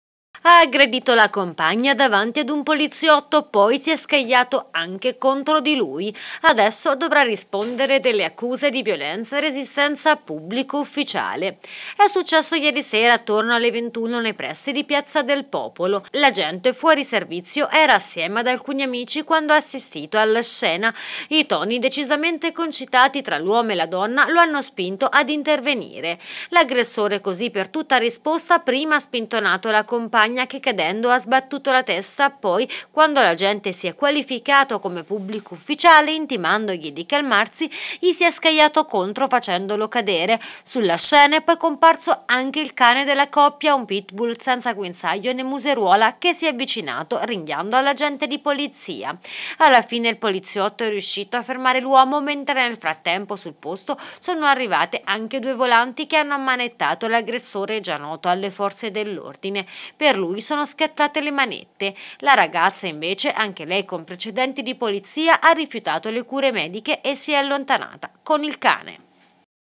Aggredisce la compagna e poi si scaglia contro un poliziotto: arrestato
Ha aggredito la compagna davanti ad un poliziotto e poi si è scagliato anche contro di lui.
Pero l'uomo sono scattate le manette. Adesso dovrà rispondere delle accuse di violenza e resistenza a pubblico ufficiale.
E' successo ieri sera attorno alle 21 nei pressi di Piazza del Popolo.